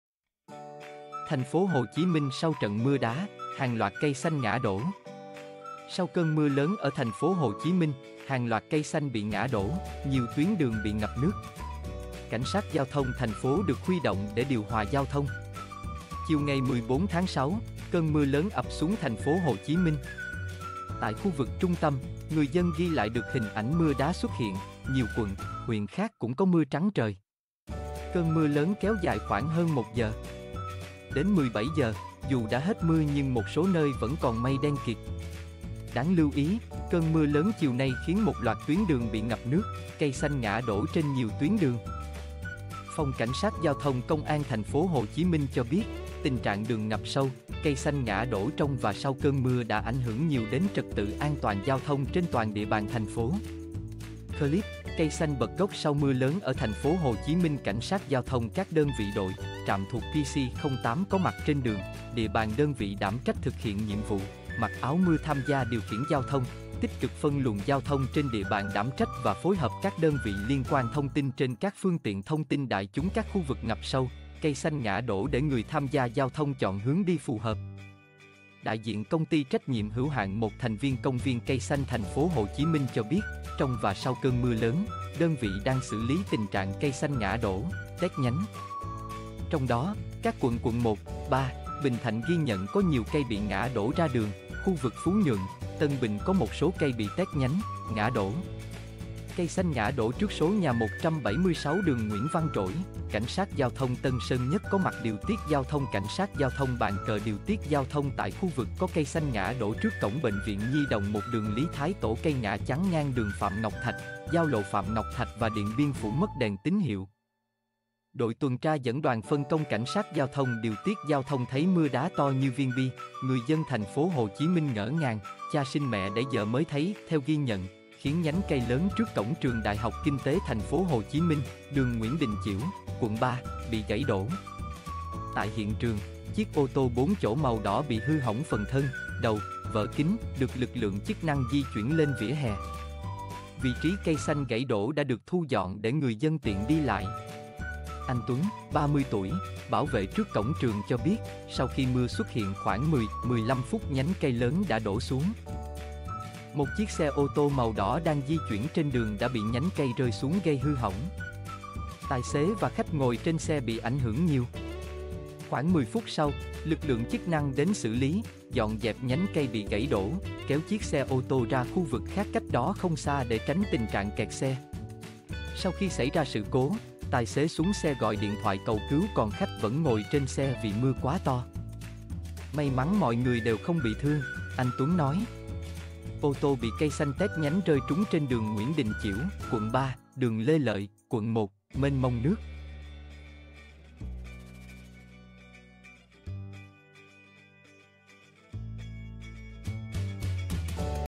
TP.HCM sau trận mưa đá: Hàng loạt cây xanh ngã đổ
Sau cơn mưa lớn ở TP.HCM, hàng loạt cây xanh bị ngã đổ, nhiều tuyến đường bị ngập nước. CSGT TP được huy động để điều hòa giao thông.

Chiều 14.6, cơn mưa lớn ập xuống TP.HCM. Tại khu vực trung tâm, người dân ghi lại được hình ảnh mưa đá xuất hiện, nhiều quận, huyện khác cũng có mưa trắng trời. Cơn mưa lớn kéo dài khoảng hơn 1 giờ. Đến 17 giờ, dù đã hết mưa nhưng một số nơi vẫn còn mây đen kịt. Đáng lưu ý, cơn mưa lớn chiều nay khiến một loạt tuyến đường bị ngập nước, cây xanh ngã đổ trên nhiều tuyến đường. Phòng CSGT Công an TP.HCM cho biết, tình trạng đường ngập sâu, cây xanh ngã đổ trong và sau cơn mưa đã ảnh hưởng nhiều đến trật tự an toàn giao thông trên toàn địa bàn TP.